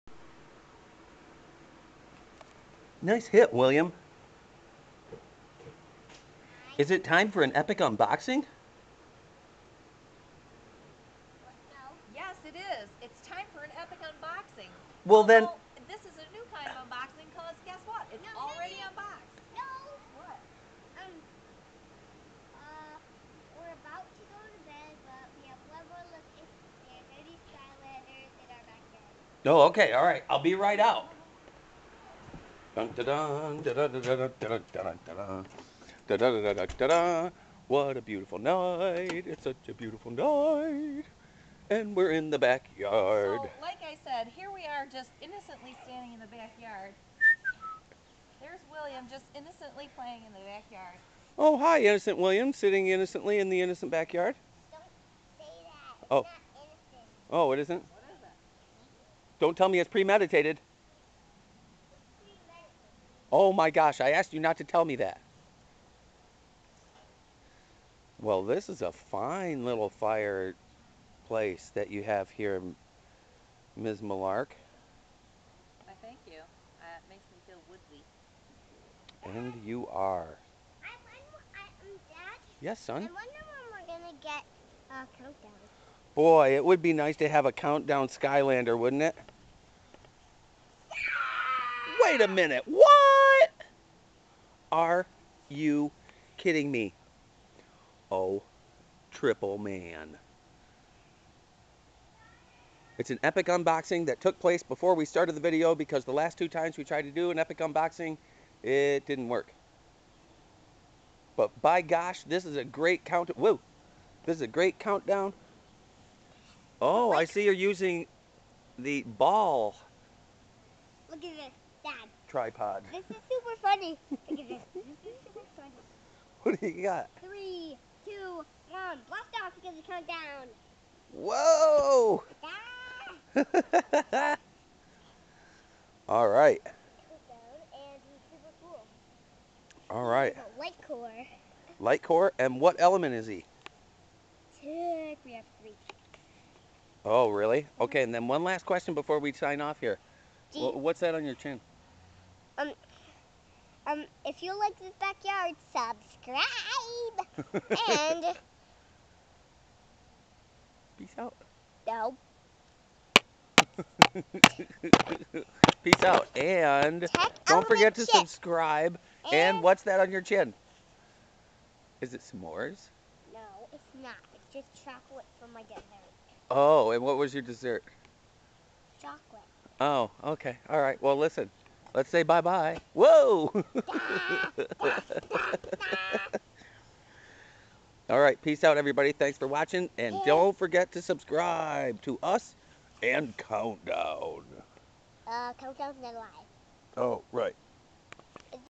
OhMyGoshTV Skylander Swap Force Epic(post) Unboxing- Countdown
Epic unboxings are maybe the coolest thing you can do on YouTube. It is critical, though, that you get the toy out (un) of the box(ing). This has been a challenge for us. We've found a new way around an age-old problem.